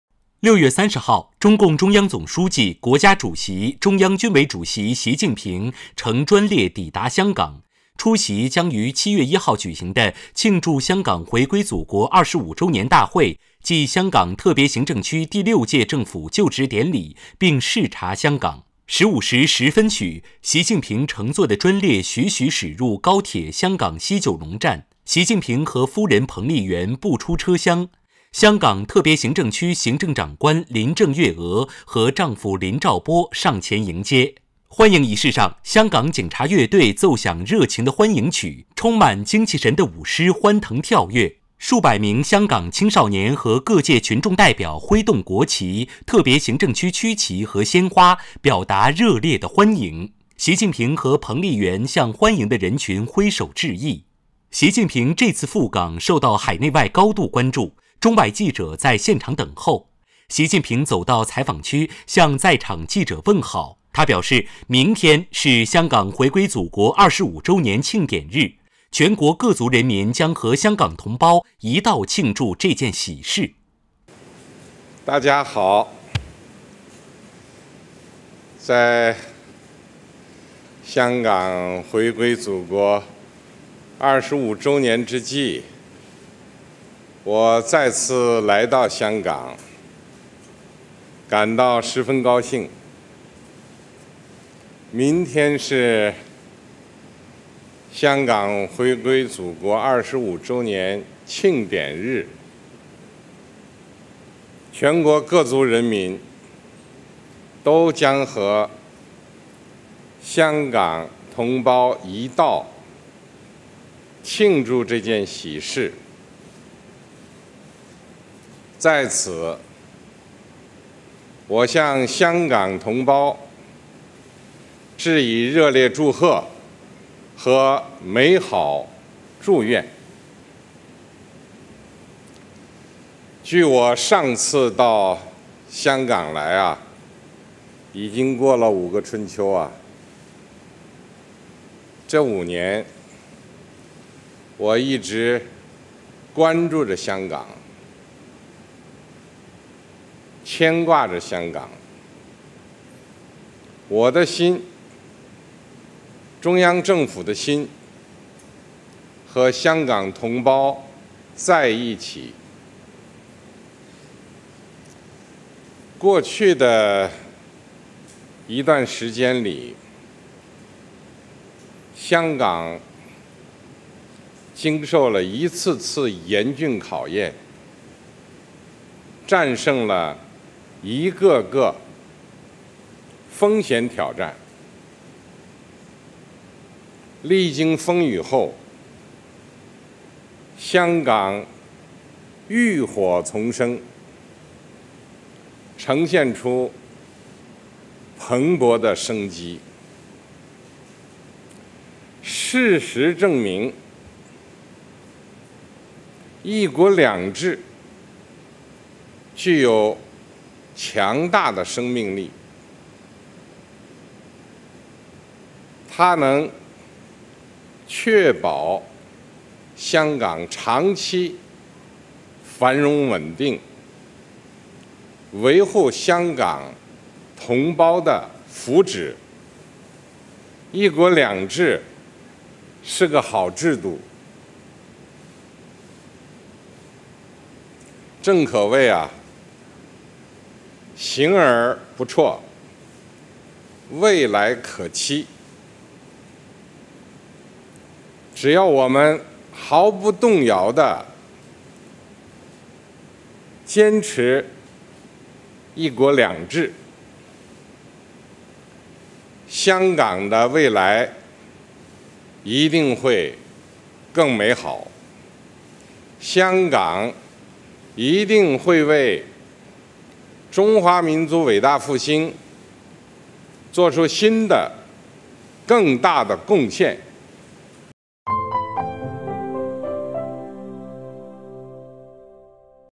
习近平：香港浴火重生 展现出蓬勃的生机
6月30日，中共中央总书记、国家主席、中央军委主席习近平乘专列抵达香港，出席将于7月1日举行的庆祝香港回归祖国25周年大会暨香港特别行政区第六届政府就职典礼，并视察香港。
         15时10分许，习近平乘坐的专列徐徐驶入高铁香港西九龙站。习近平和夫人彭丽媛步出车厢，香港特别行政区行政长官林郑月娥和丈夫林兆波上前迎接。
         欢迎仪式上，香港警察乐队奏响热情的欢迎曲，充满精气神的舞狮欢腾跳跃，数百名香港青少年和各界群众代表挥动国旗、特别行政区区旗和鲜花，表达热烈的欢迎。习近平和彭丽媛向欢迎的人群挥手致意。
         习近平这次赴港受到海内外高度关注，中外记者在现场等候。习近平走到采访区，向在场记者问好。他表示，明天，是香港回归祖国25周年庆典日，全国各族人民将和香港同胞一道庆祝这件喜事。